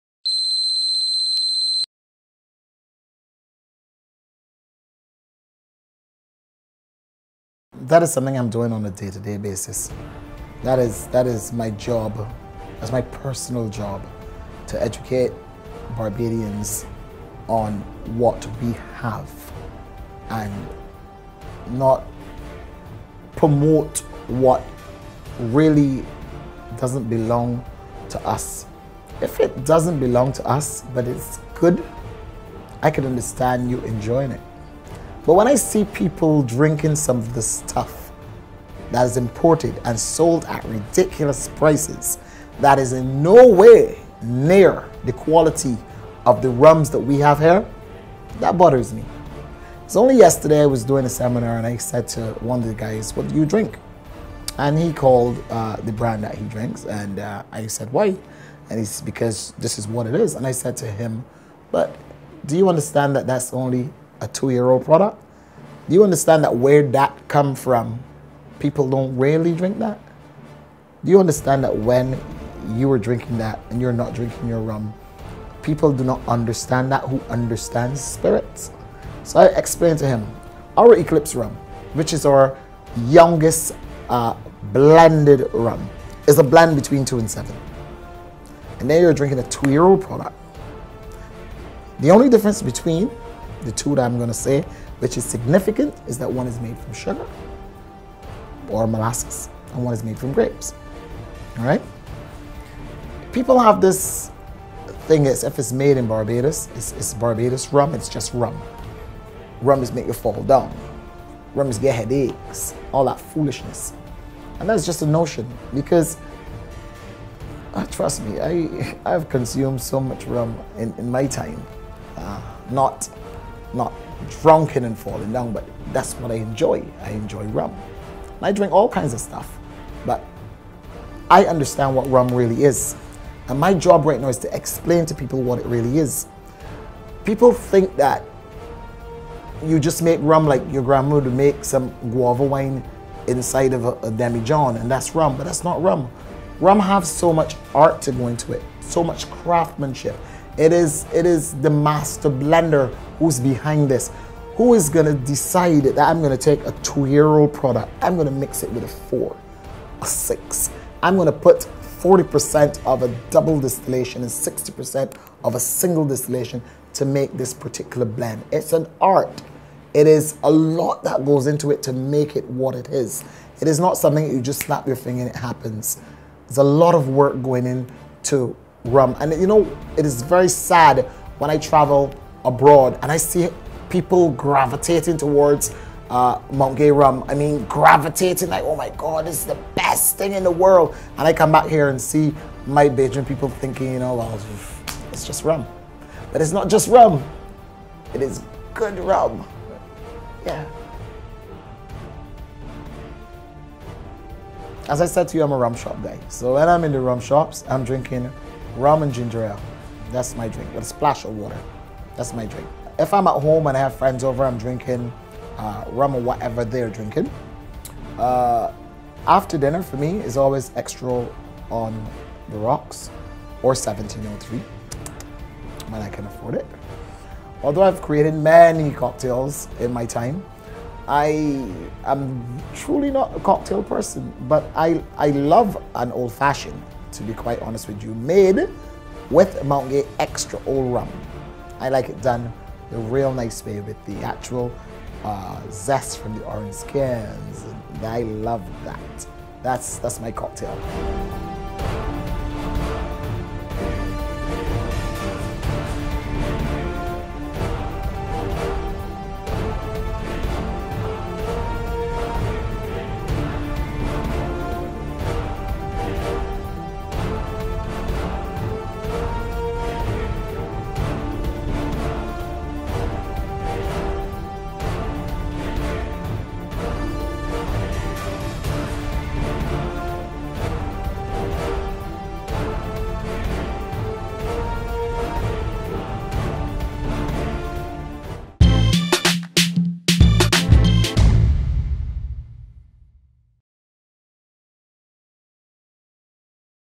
CHESTER BROWNE -Interview part 2
Chester Brown tells us how he truly feels about RUM.